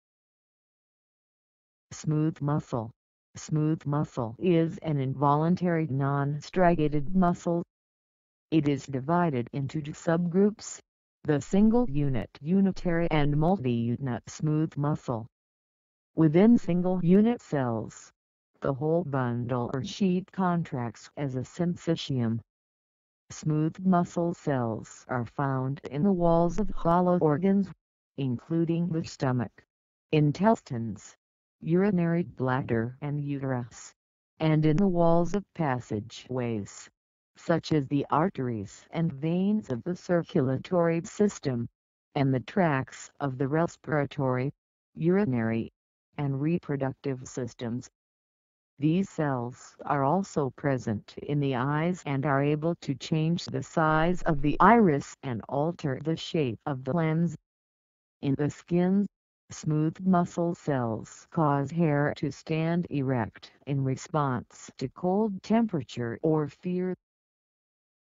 What Is SMOOTH MUSCLE? SMOOTH MUSCLE Definition & Meaning
What is SMOOTH MUSCLE, What does SMOOTH MUSCLE mean, SMOOTH MUSCLE meaning, SMOOTH MUSCLE definition, SMOOTH MUSCLE explanation

Smooth muscle is an involuntary non-striated muscle. It is divided into two subgroups; the single-unit (unitary) and multiunit smooth muscle. Within single-unit cells, the whole bundle or sheet contracts as a syncytium.

Smooth muscle cells are found in the walls of hollow organs, including the stomach, intestines, urinary bladder and uterus, and in the walls of passageways, such as the arteries and veins of the circulatory system, and the tracts of the respiratory, urinary, and reproductive systems. These cells are also present in the eyes and are able to change the size of the iris and alter the shape of the lens. In the skin, smooth muscle cells cause hair to stand erect in response to cold temperature or fear.[1]
Source: Wikipedia.org